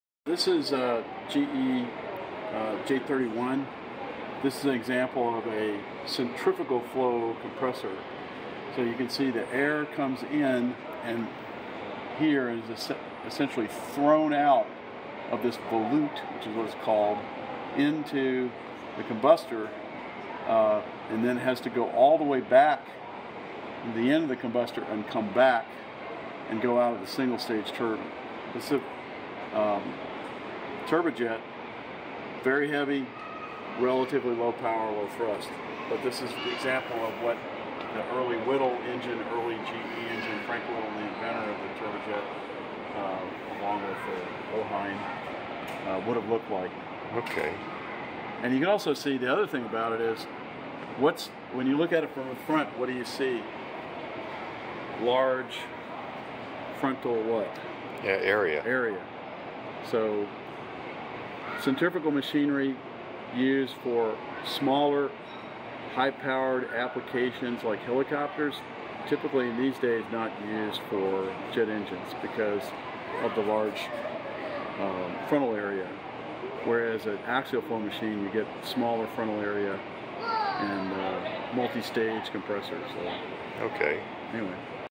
GE J-31 Centrifugal Turbojet Explained, Second World War
Museum docent explains the workings of the J-31